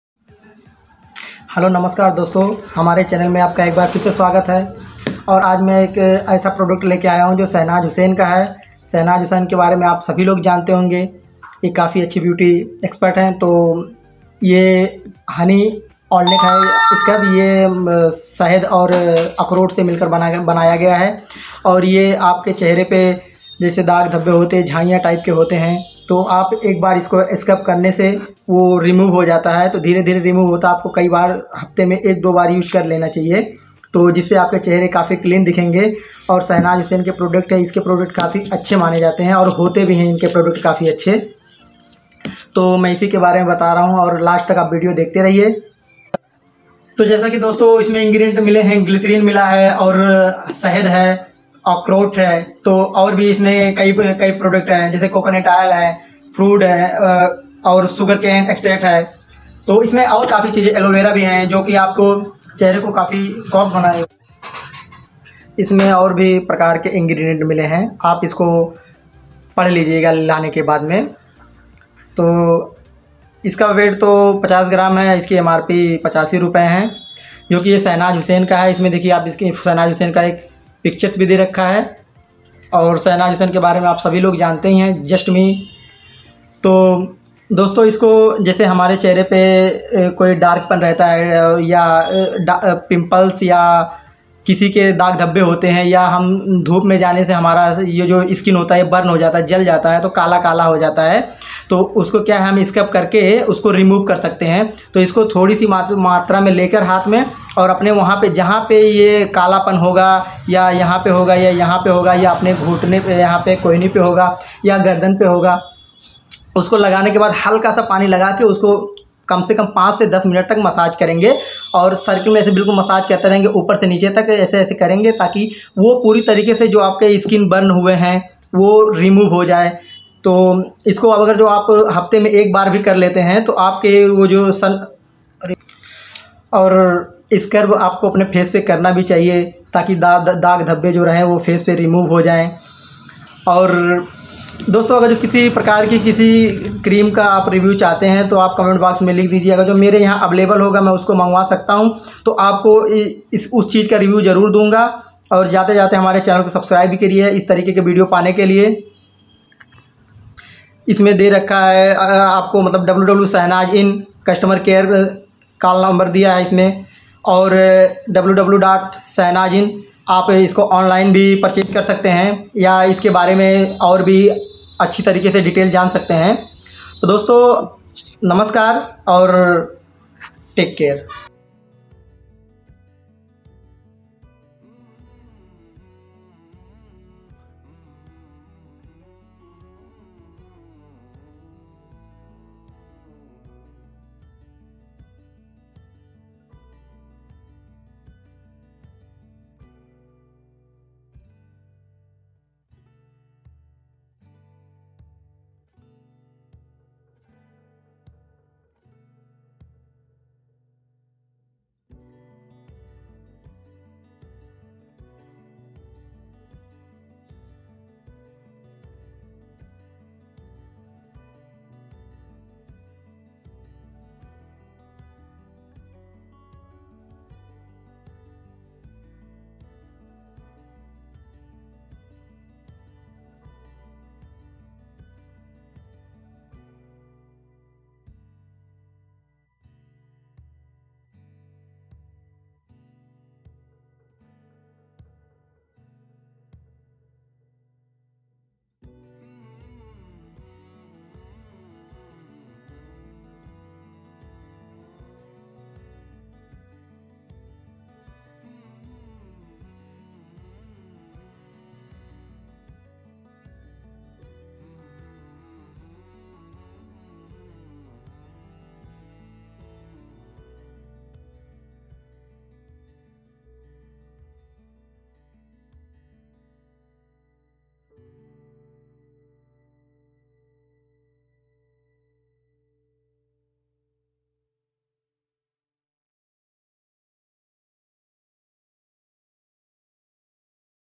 Just ME Shahnaz Husain Honey walnut scrub
Just me honey walnut scrub by Shahnaz Husain
Net weight 50 gram
Doston yah face scrub Shahnaz Husain ki hai hai jo ki kafi achcha scrub hai isko hafte mein main ek ya do baar lagan se a aapke chehre per glowing aur chehra Sundar dikhega.
Is scrub ko ayurvedic tarike se banaya gaya hai.